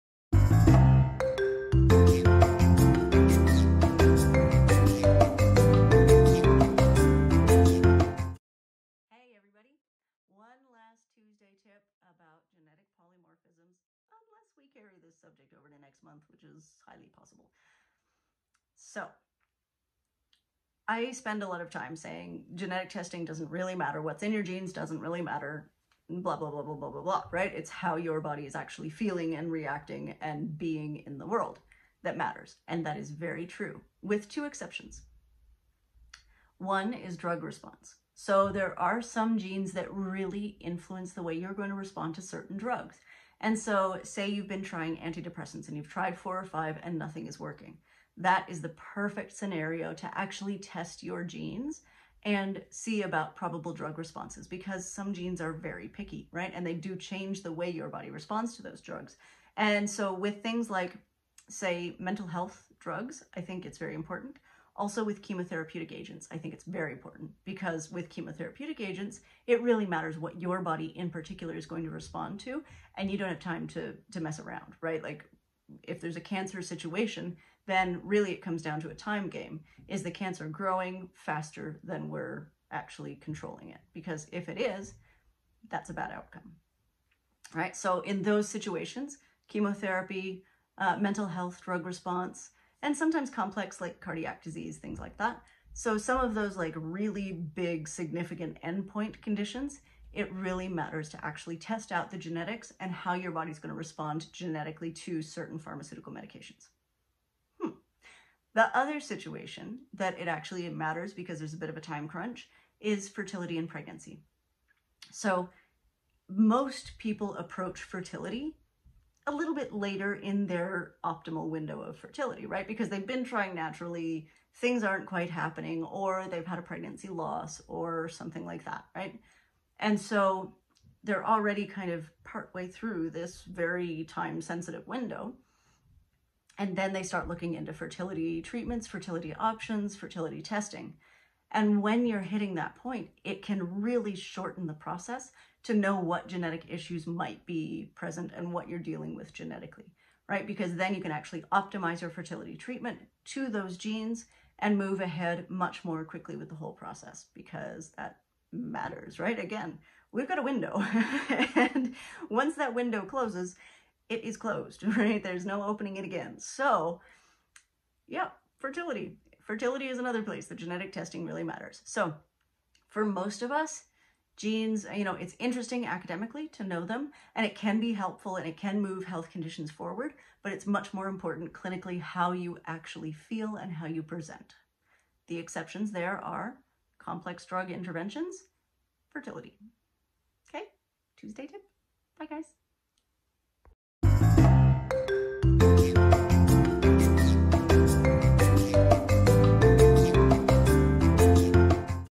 TT #32 Two times that genetic testing really matters
Most of the time, how you feel and how your symptoms are doing is what matters most.  There are two scenarios in which genetic testing does matter. Let's talk about them!